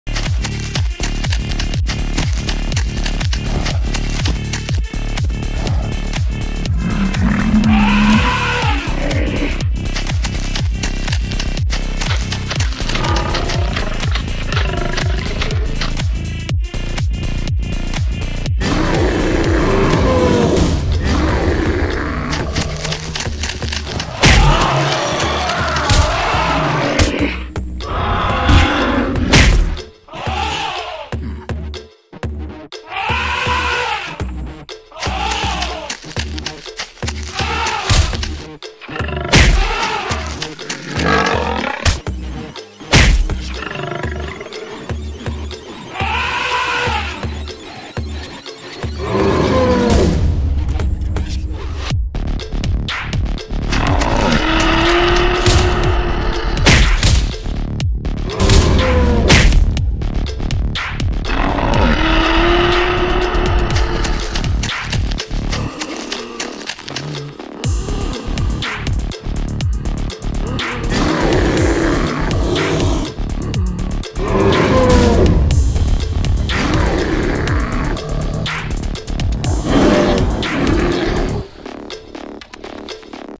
Mammoth & Lion King vs zombie Elephant & Rhinoceros
African zombie Elephant and Rhinoceros defend their territory
and infect invaders,Lion King and Mammoth fought with dignity
but lost and got infected with Zombie virus